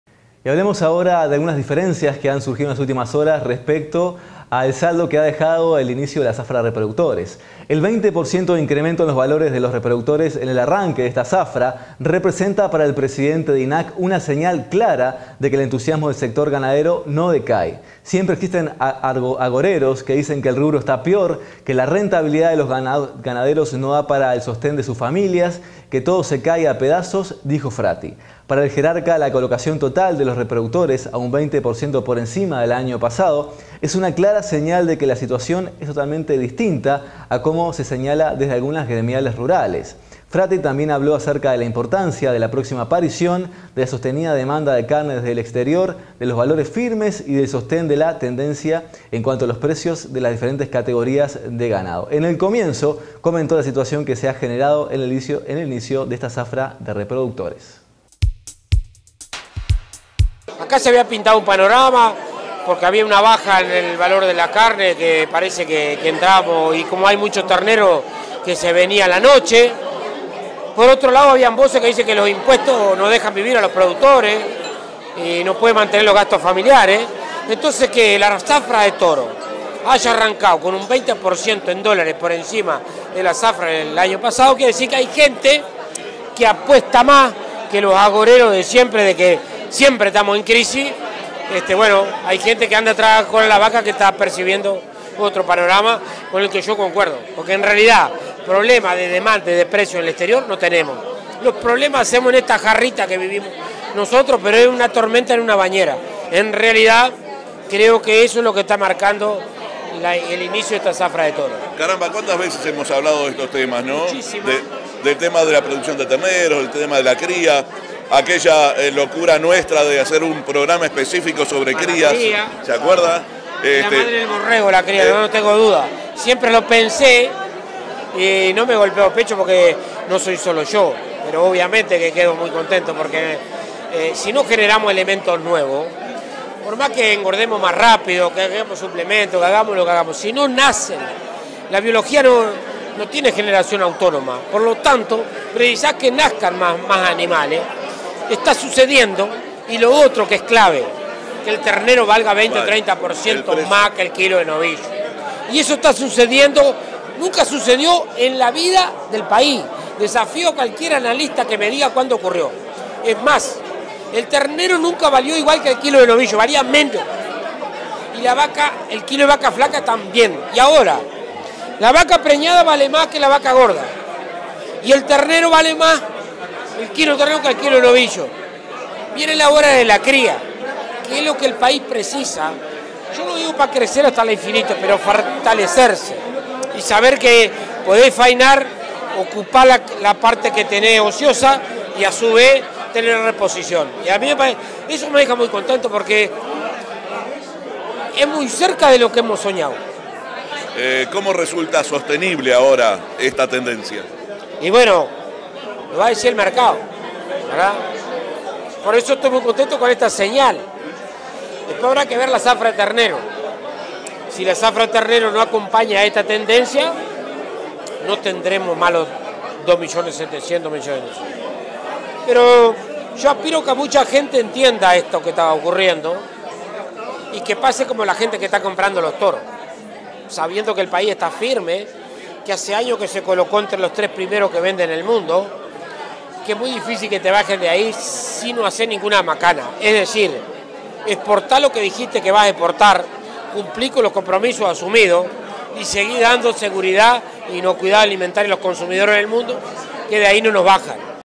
ALFREDO FRATTI
EL PRESIDENTE DE INAC, ADJUDICÓ LA BUENA COLOCACIÓN DE TOROS AL MOMENTO ECONOMICO FAVORABLE DE LA GANADERÍA, SALIENDO AL PASO A OPINIONES QUE DICEN LO CONTRATRIO.